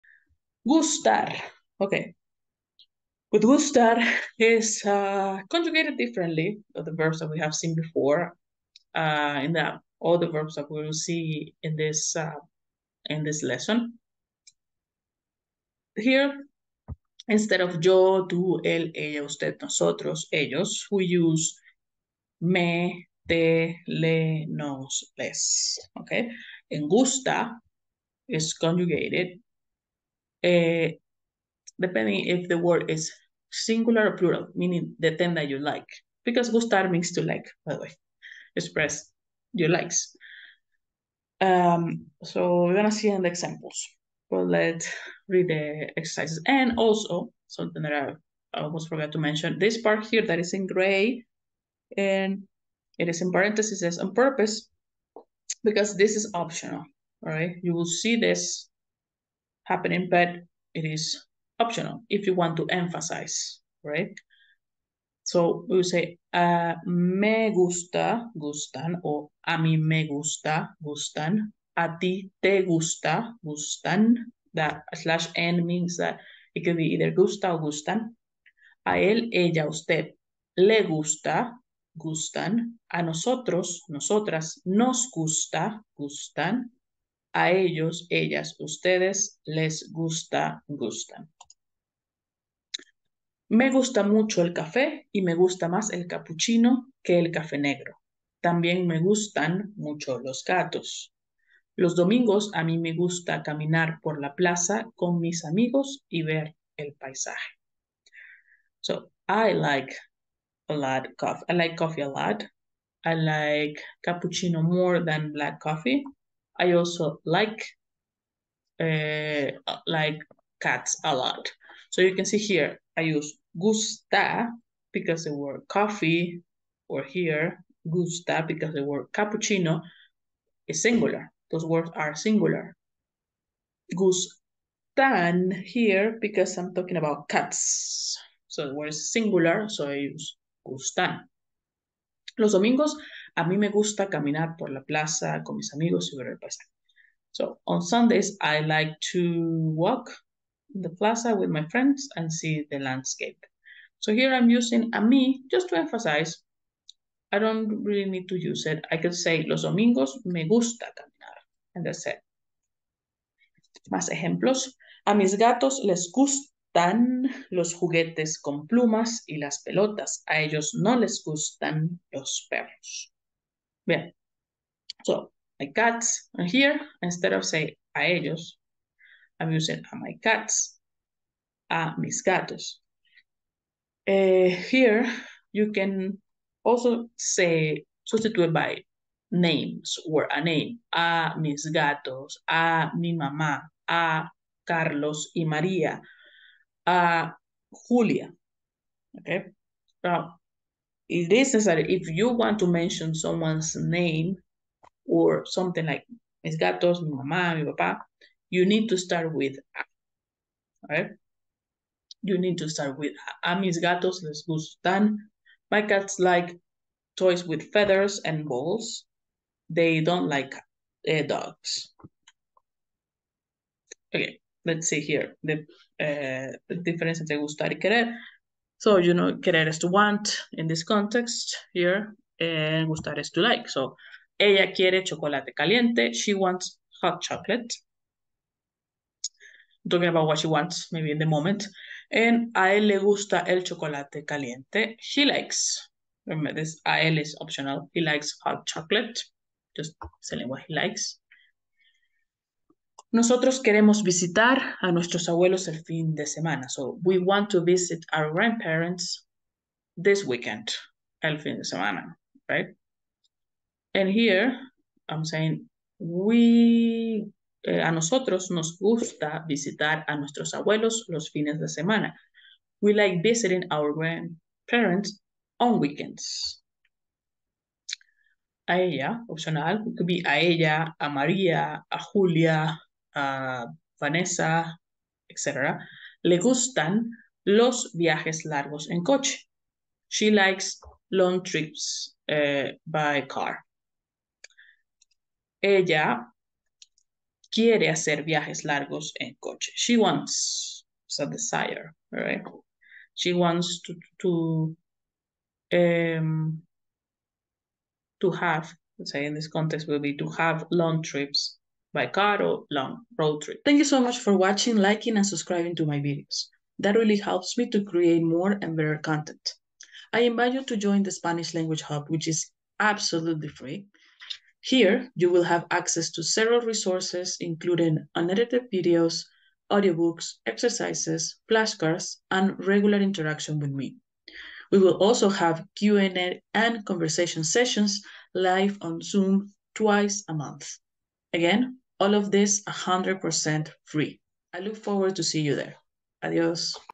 ESSENTIAL VERBS IN SPANISH #9 - GUSTAR
Hey there! 👋 Ever wondered how to use the Spanish verb 'Gustar'? 🇪🇸 In this video, we'll break it down step by step, so you can express what you like and don't like in Spanish! 🌟 Join me for a fun and simple explanation! 🗣️👇 Let's get started! 💃🕺

*Join my FREE Google Classroom for worksheets, interactive activities, audios, transcripts, and live classes

*Join my Spanish Conversation Club or become a supporter and choose the topics of my videos